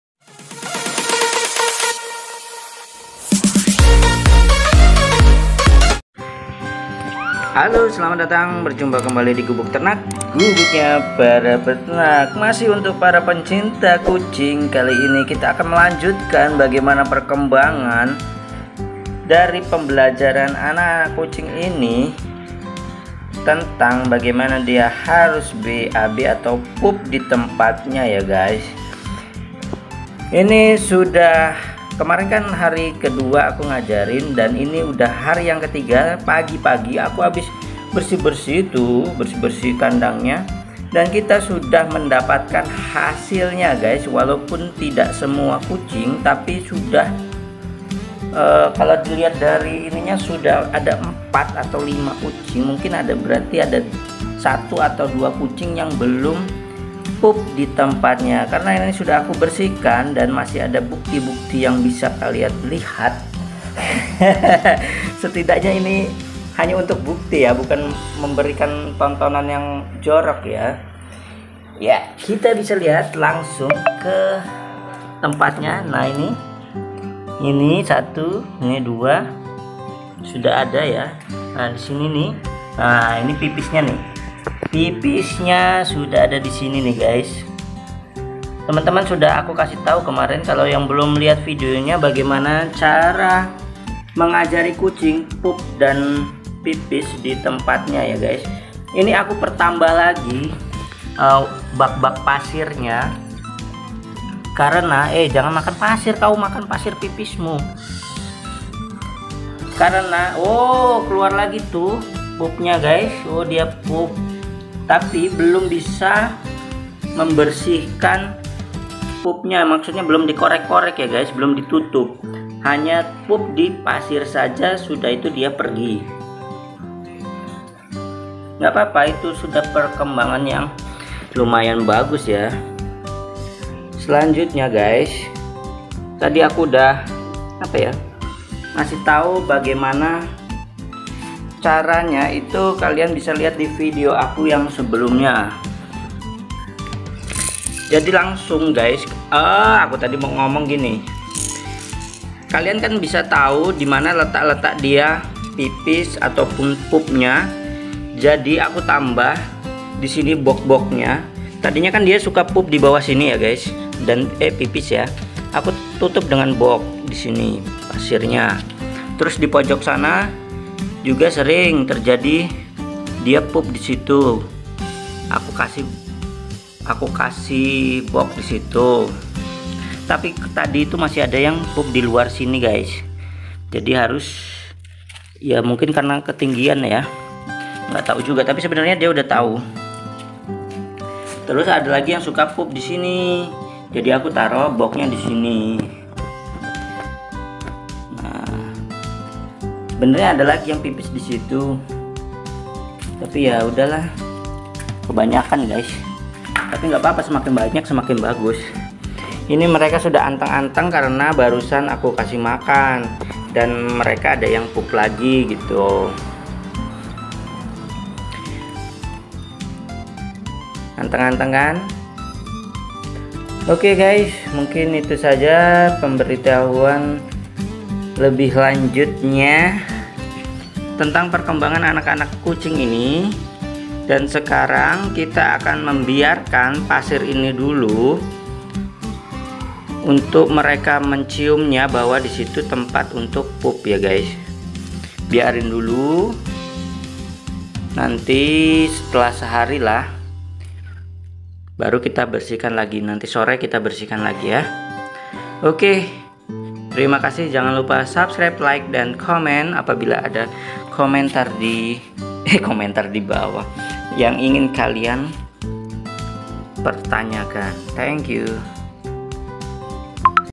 Berhasil‼️ Cara Mengajarkan Anak Kucing Pup dan Pipis Di Bak Pasir
akali ini kita akan lanjutkan perkembangan cara mengajarkan anak kucing pup dan pipis di bak pasir.. Dari video sebelumnya ternyata hasilnya sangat memuaskan dan cara ini benar benar berhasil